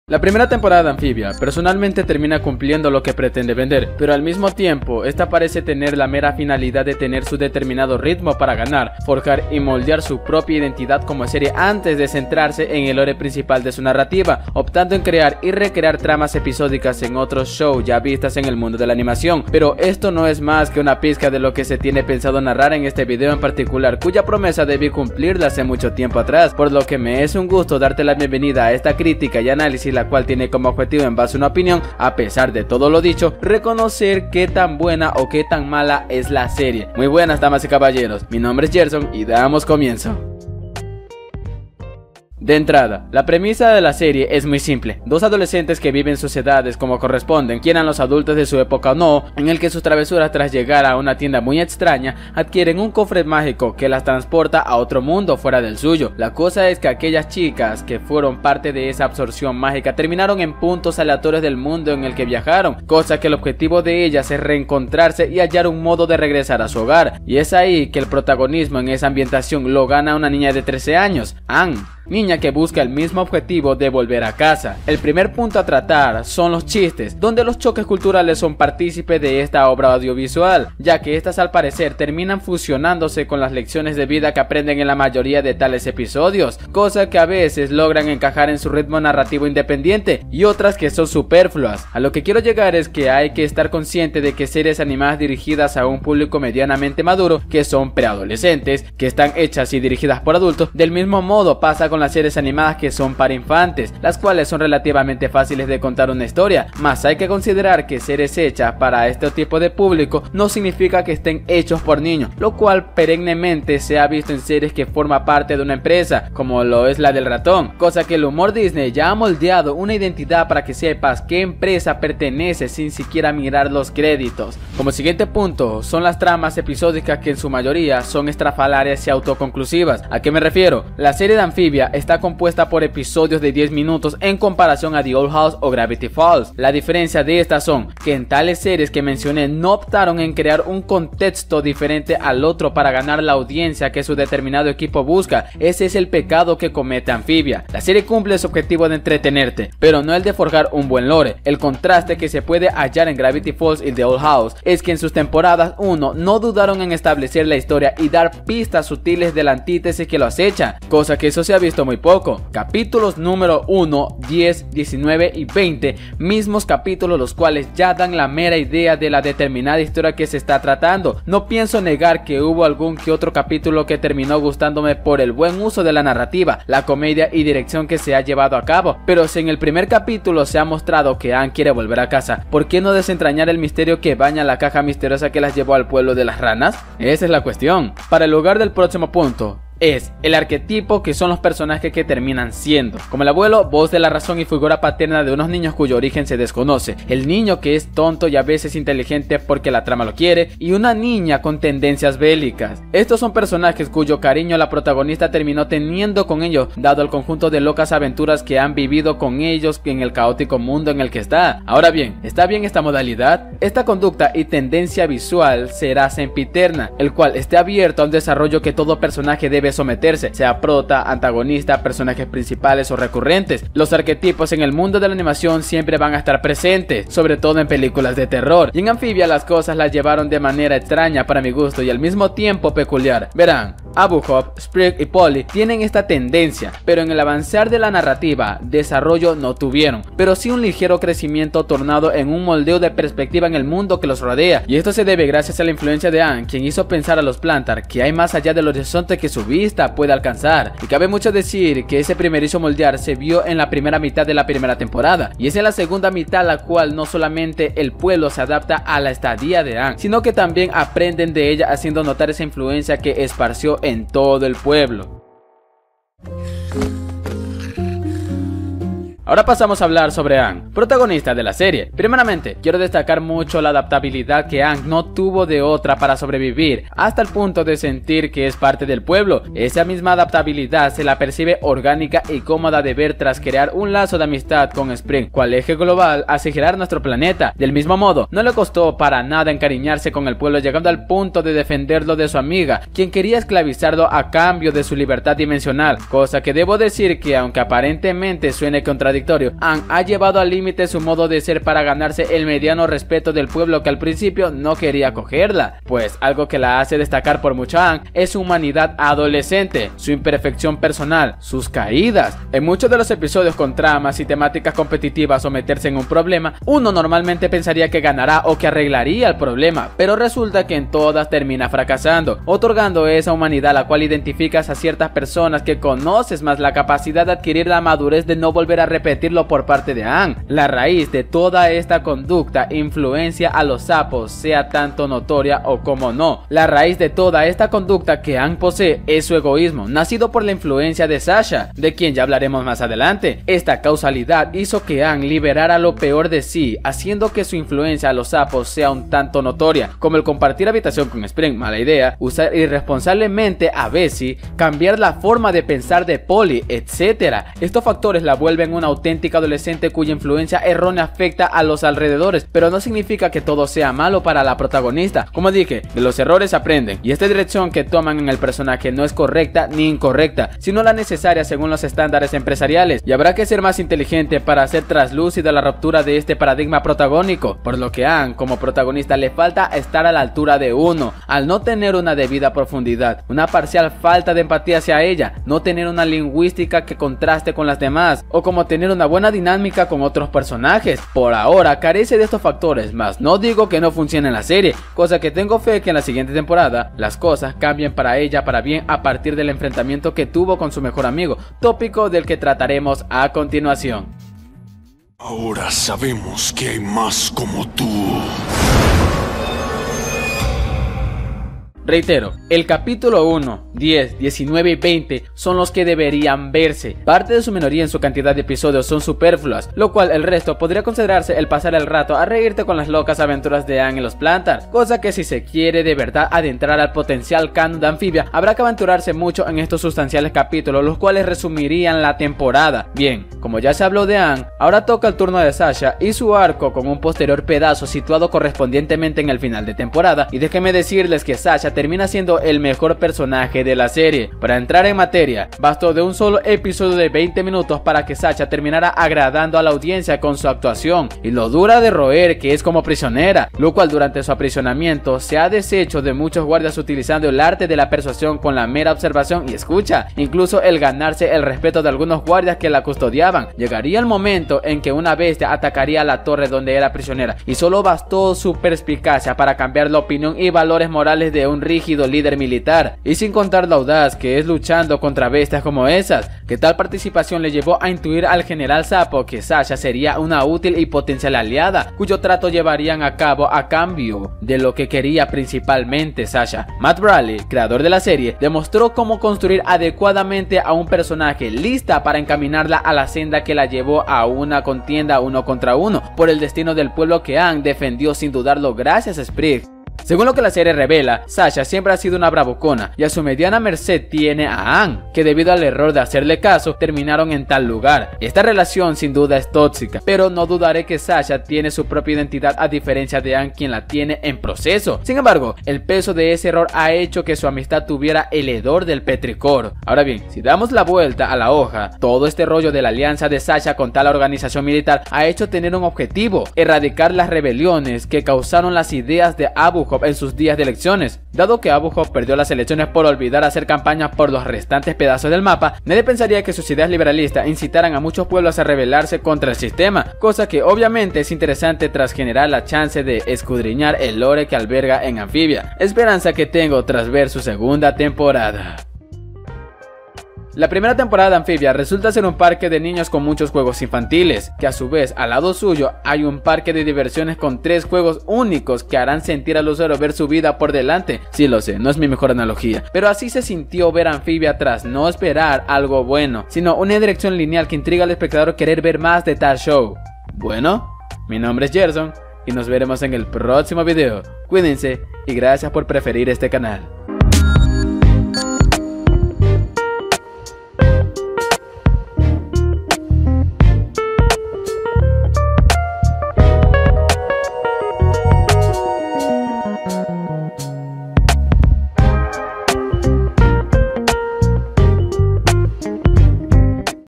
Amphibia Parte 1: Un Parque de Diversiones en el que tú decides Disfrutar
Crítica y Análisis de la Primera Temporada de Amphibia, dividido en partes que serán desentrañadas en el transcurso de este video.

Capítulos
0:00 - Introducción
0:41 - Breve Crítica
4:25 - Anne
6:44 - Children of the Spore
9:10 - Conclusión

Twitter (En el que suelo decir cosas aleatorias, y también respecto al futuro del canal)